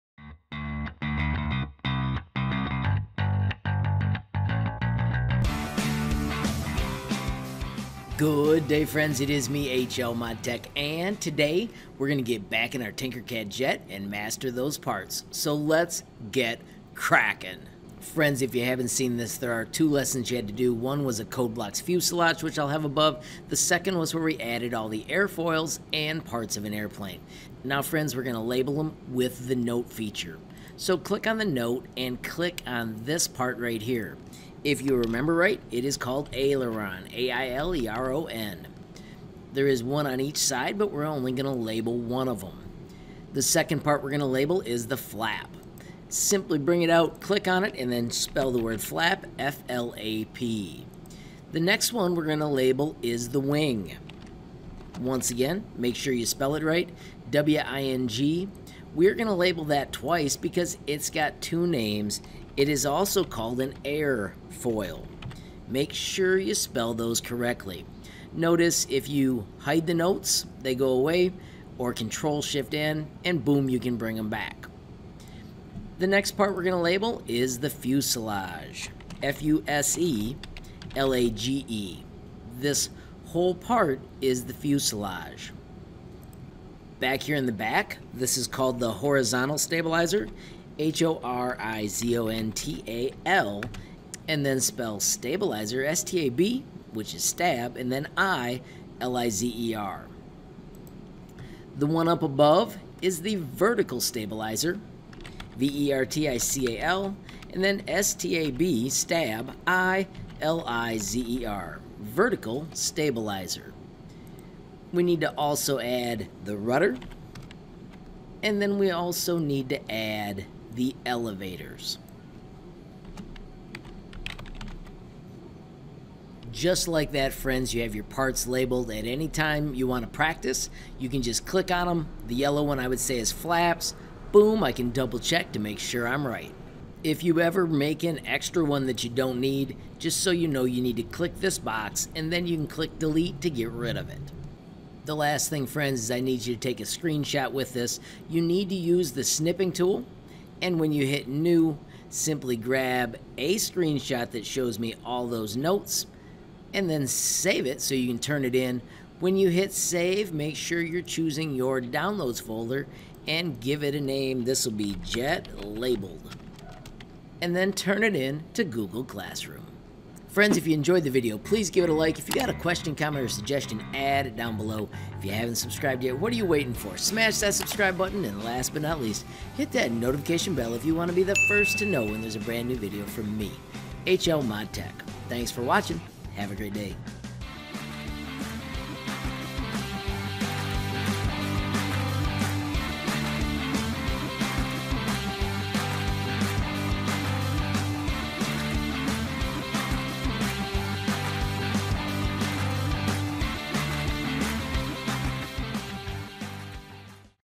Use Tinkercad Notes to label your Jet Airliner Parts in Minutes
Have a glorious day and keep Tinkering! Master the parts of your airliner by using Tinkercad Notes to label your Jet Airliner in Minutes. A slick way to almost make flashcards about the parts of your airplane.